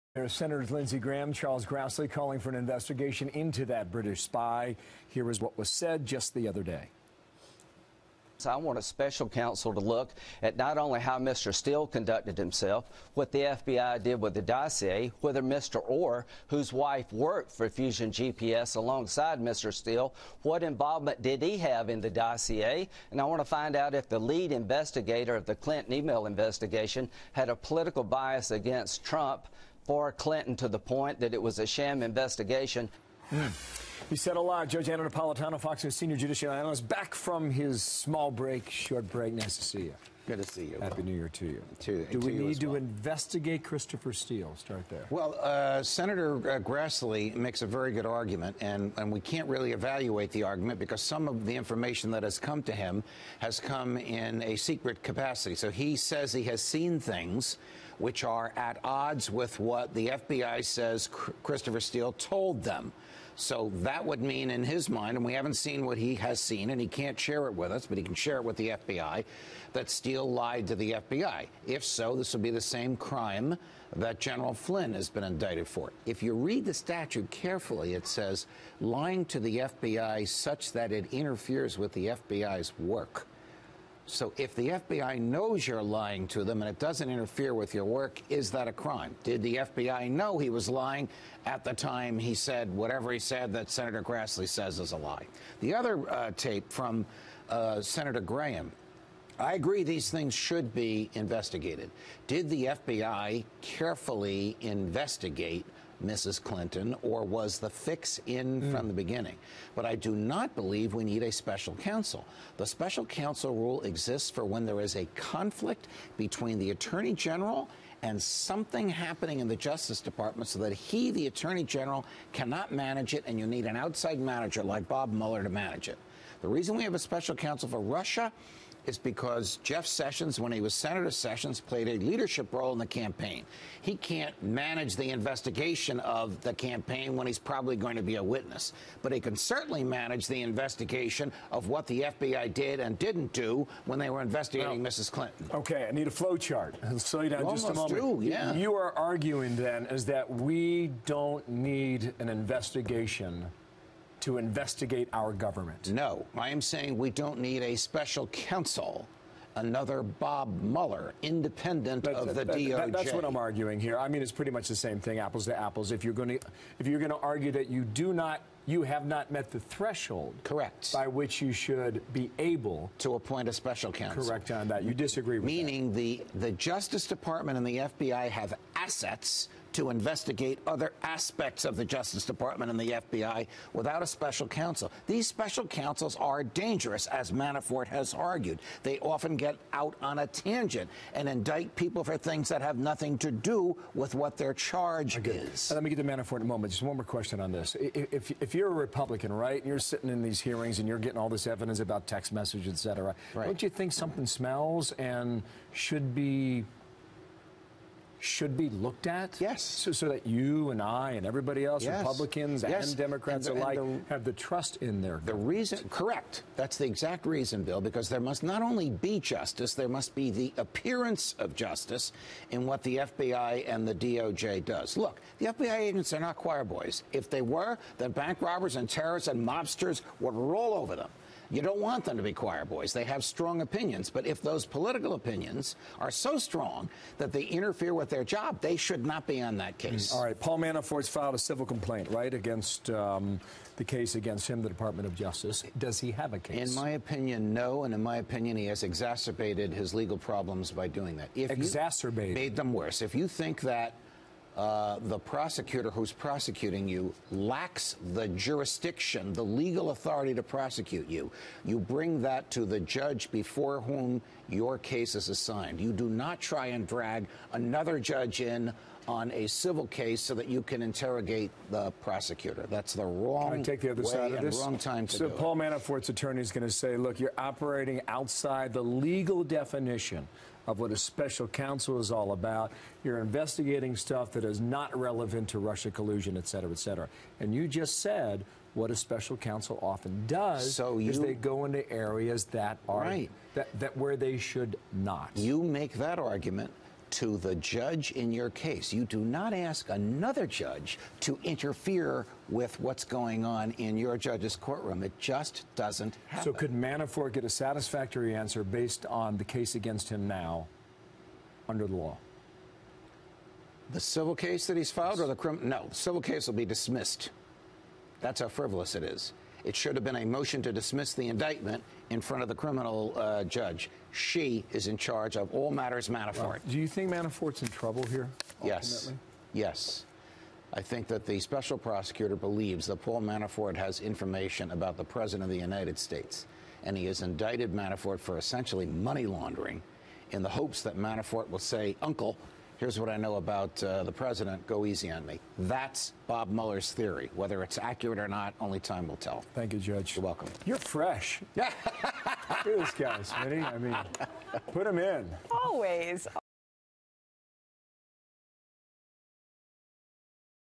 Judge Napolitano | Mueller Believes Manafort Will Roll Over on Pres. Trump
"I think that the special prosecutor believes that Paul Manafort has information about the president."